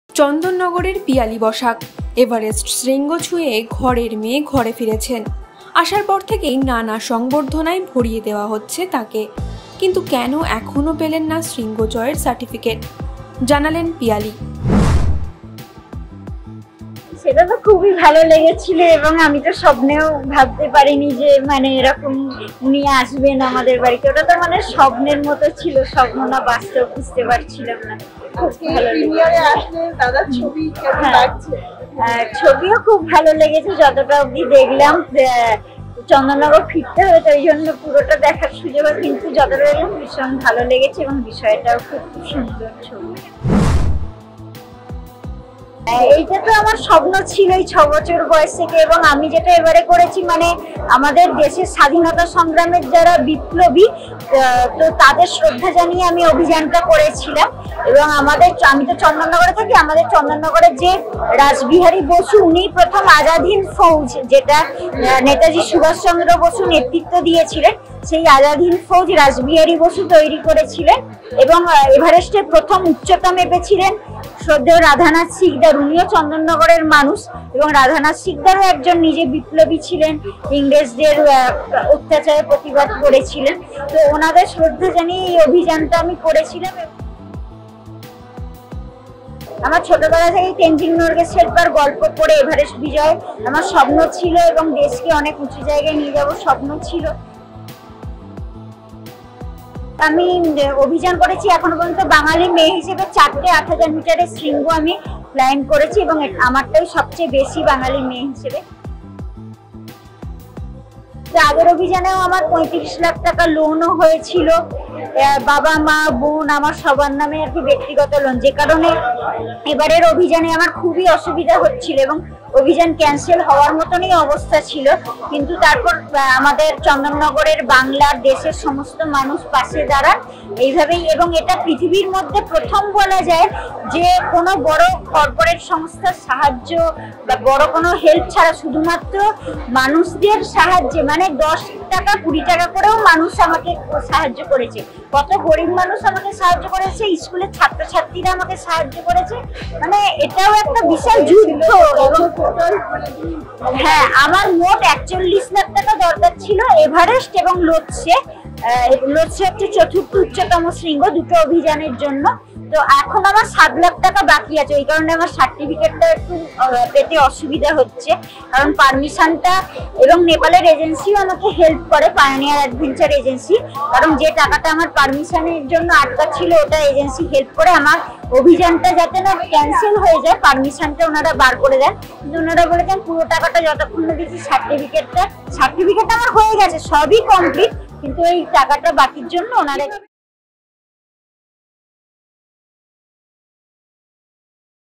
এভারেস্ট জয় করেও কেন শংসাপত্র পেলেন না পিয়ালী? | See what climber Piyali Basak says | Ei Samay
চন্দননগরের পিয়ালী বসাক। এভারেস্ট শৃঙ্গ ছুঁয়ে ঘরের মেয়ে ঘরে ফিরেছেন। আসার পর থেকেই নানা সংবর্ধনায় ভরিয়ে দেওয়া হচ্ছে তাঁকে। কিন্তু কেন এখনও পেলেন না শৃঙ্গ জয়ের সার্টিফিকেট? জানালেন পিয়ালী।